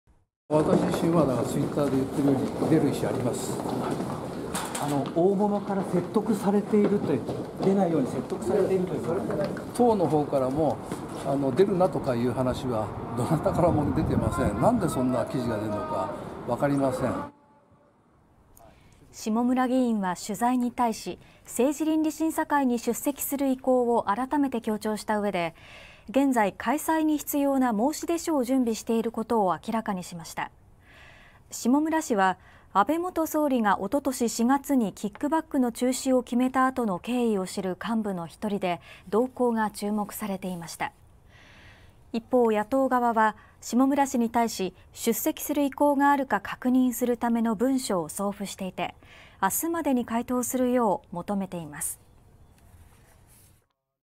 「出る意思はある」自民・安倍派の下村博文元文科大臣 政倫審出席の意向を改めて強調「出るなという指示もない」 | TBS NEWS DIG
自民党の派閥の裏金事件をめぐって、安倍派で事務総長を務めた下村博文元文部科学大臣は政治倫理審査会に「出席する意思はある」と明言しました。

自民党・安倍派　下村博文元文科大臣
「私自身はTwitter（X）で言ってるように、出る意思あります。（Q.大物（議員）から出ないように説得されていると）党の方からも出るなとかいう話は、どなたからも出てません。なんでそんな記事が出るのかわかりません」

下村議員は取材に対し、政治倫理審査会に出席する意向を改めて強調したうえで、現在、開催に必要な申出書を準備していることを明らかにしました。

下村氏は、安倍元総理がおととし4月にキックバックの中止を決めたあとの経緯を知る幹部の一人で、動向が注目されていました。

一方、野党側は下村氏に対し出席する意向があるか確認するための文書を送付していて、あすまでに回答するよう求めています。